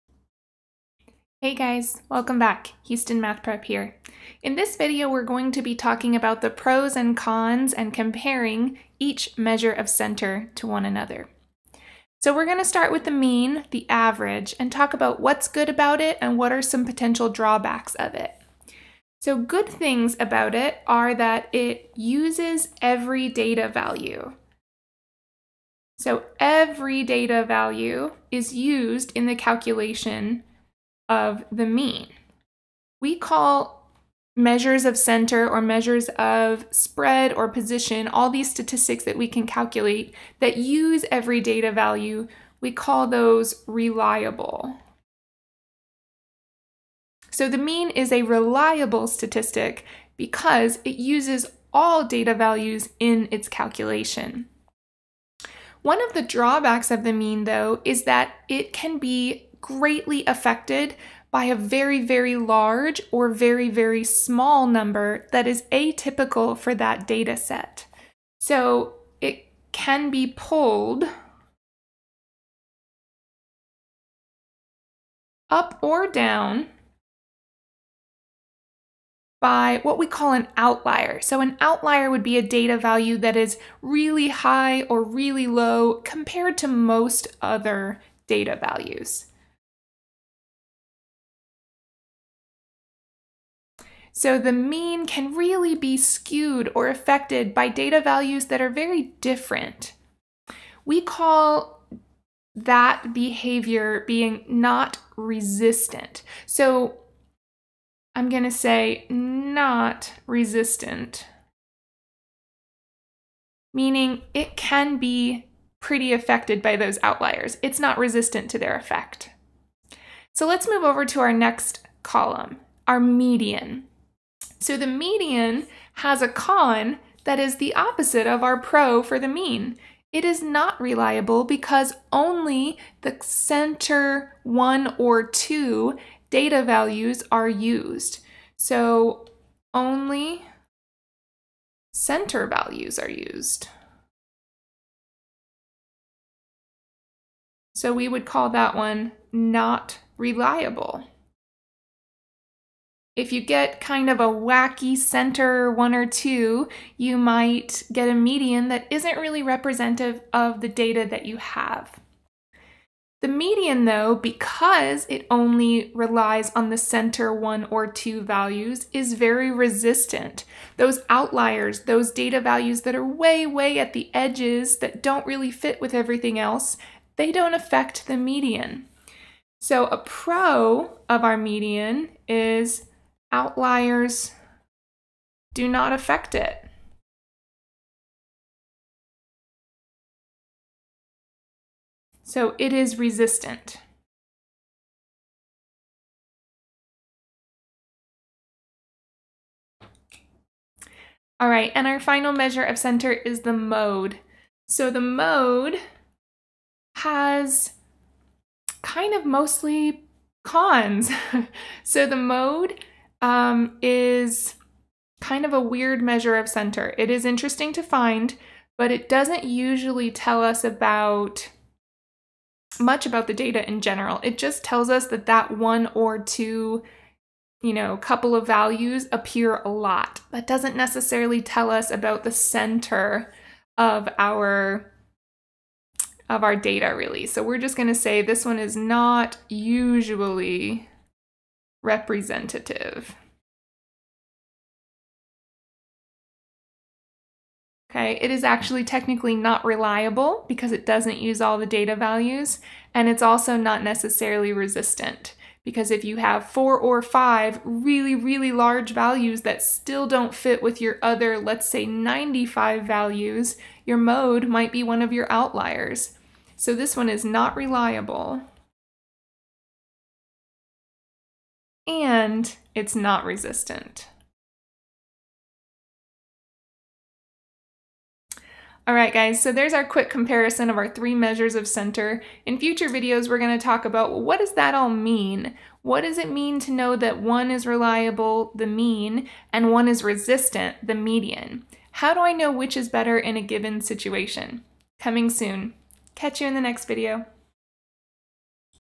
Comparing Measures of Center
Mean, Median or Mode?  The pros and cons of each measure of center and when the mean or the median is most appropriate to use